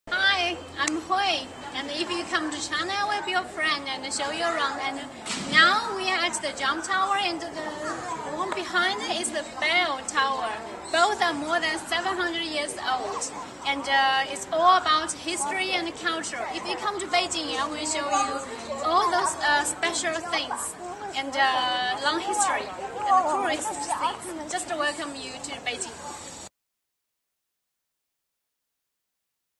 Study or intern abroad in Beijing
Hui, Panrimo's Beijing coordinator, shows you around the city.